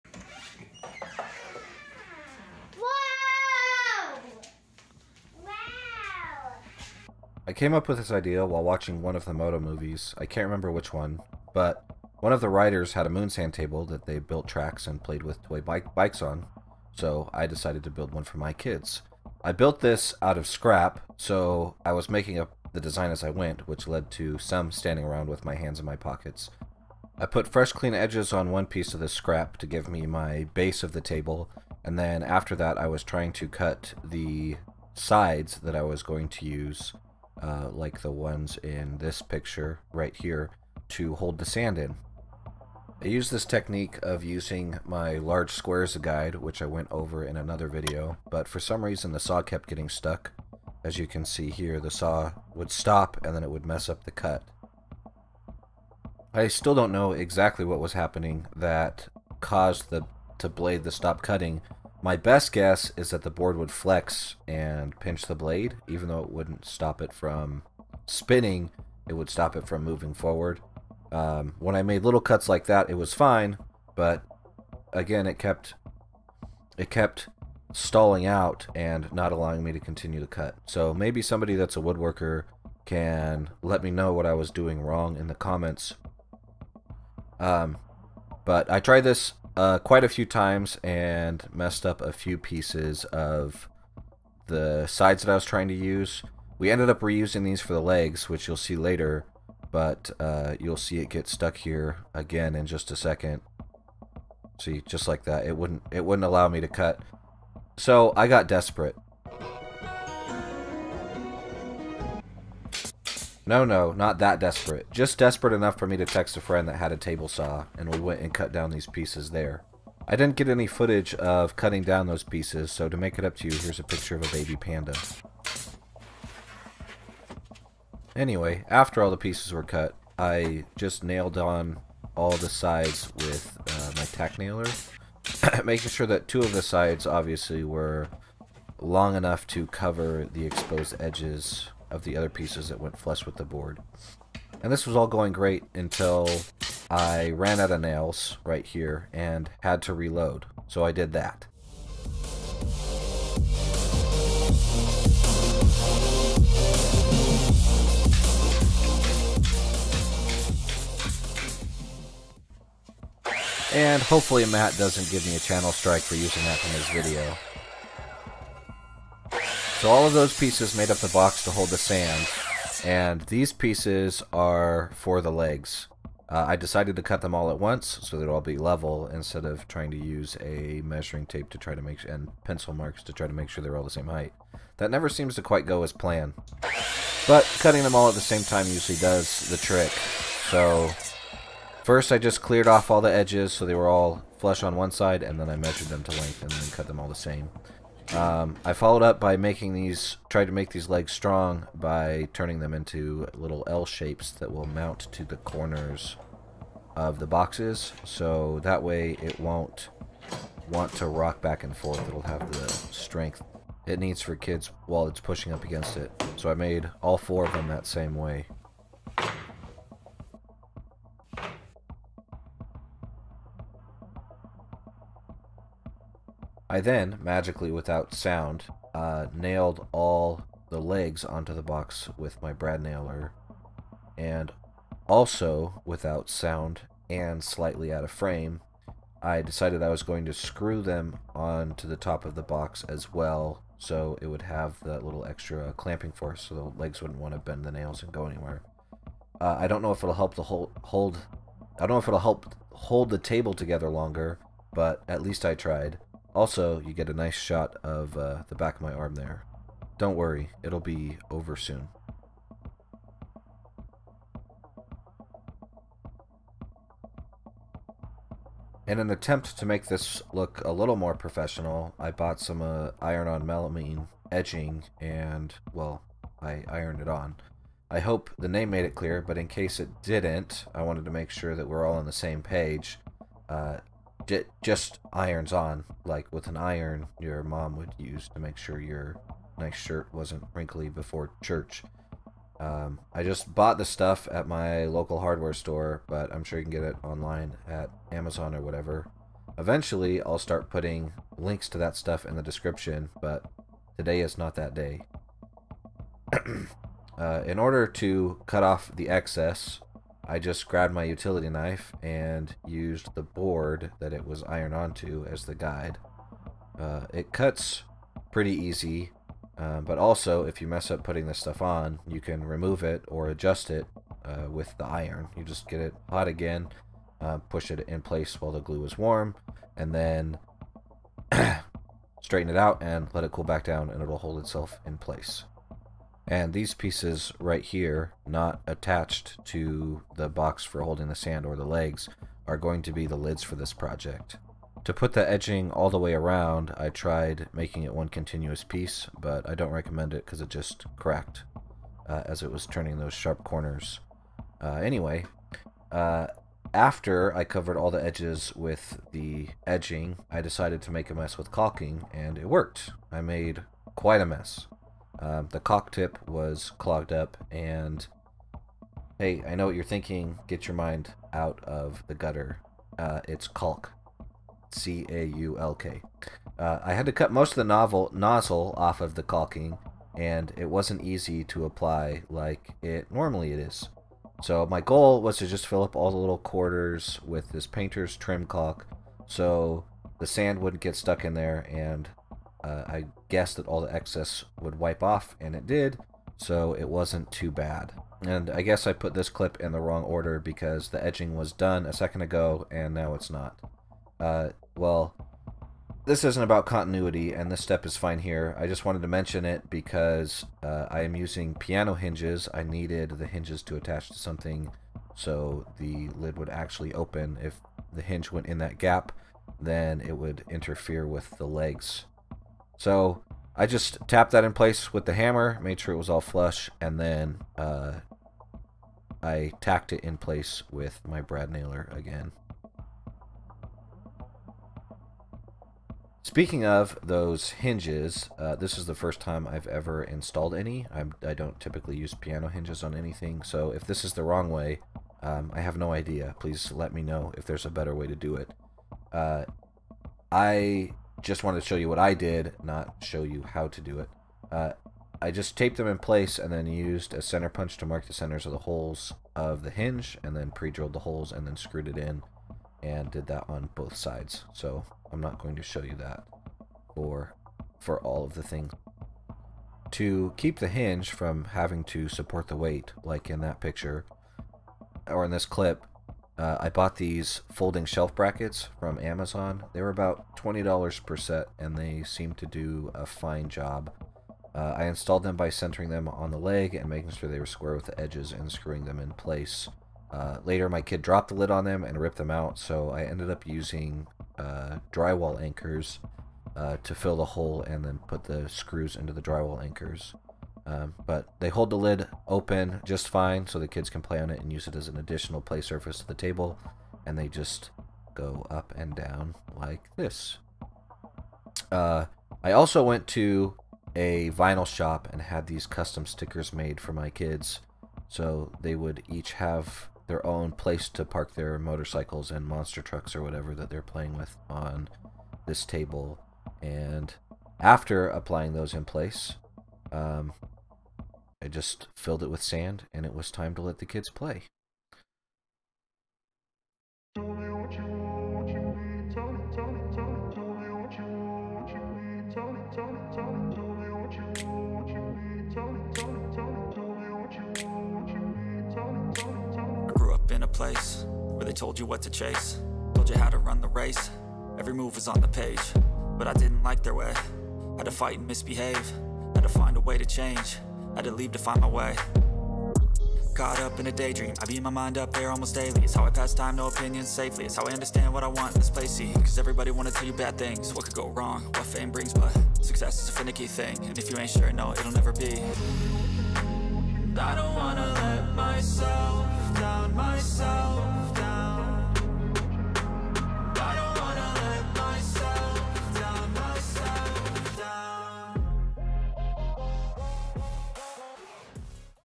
Repurposing shelves to kids toy | Full Build
My kids Toys needed a place to be played with so I made one.